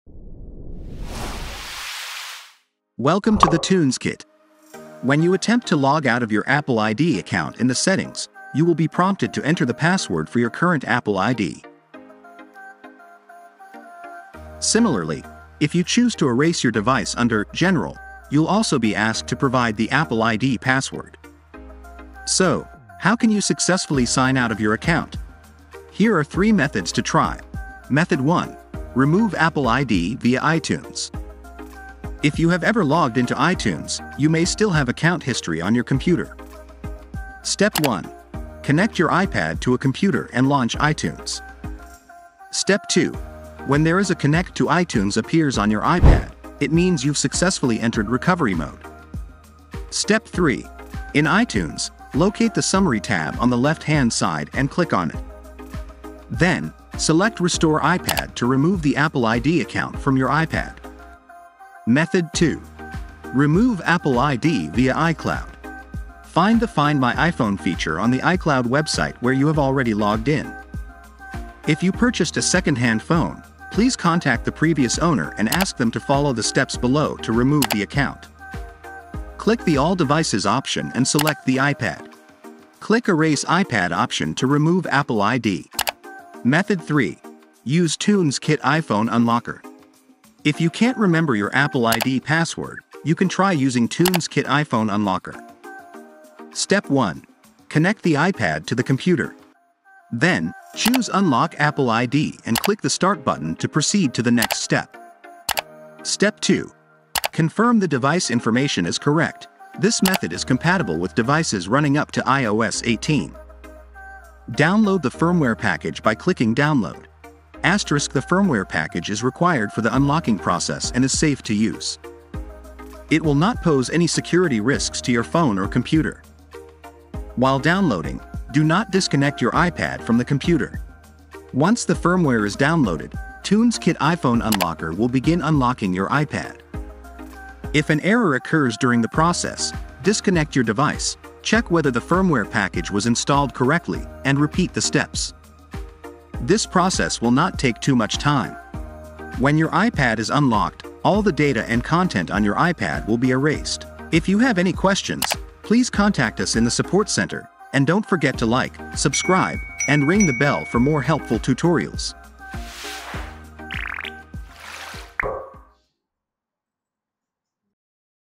How to Sign Out Apple ID on iPad without Passcode -- supported iOS 18
Hi, welcome to the TunesKit. Today's video will show you how to sign out Apple ID on iPad without password.

⏰ Timestamps:
00:00 Start
00:29 Method 1:  Remove Apple ID Via iTunes
01:02 Method 2:  Remove Apple ID Via iCloud
01:27 Method 3:  Use TunesKit iPhone Unlocker
02:49 The End

To learn more about this topic, visit the following article: 👇
3 Ways to Remove Apple ID from iPad without Password